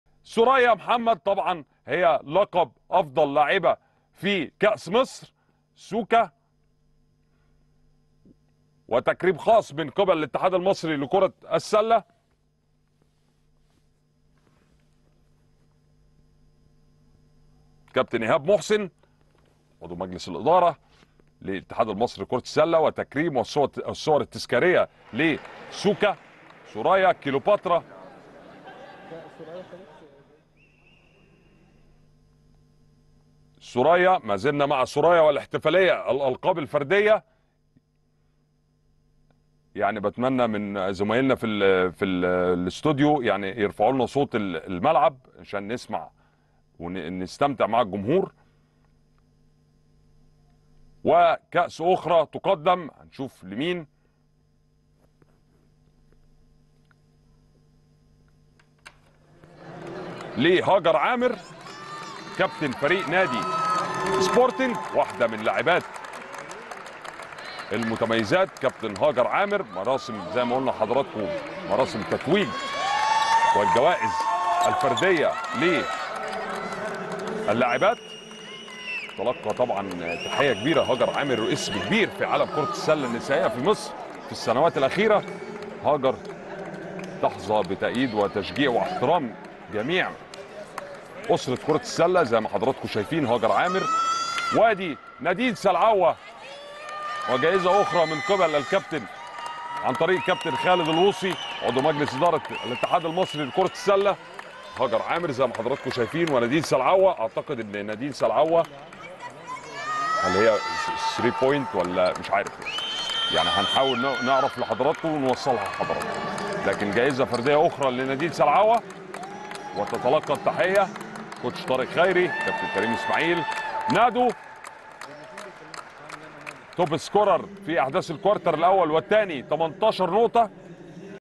شاهد.. مراسم تسليم جوائز بطولة كأس مصر لسيدات كرة السلة
مراسم التتويج  🥇 شاهد تسليم جوائز  بطولة كأس مصر لسيدات كرة السلة.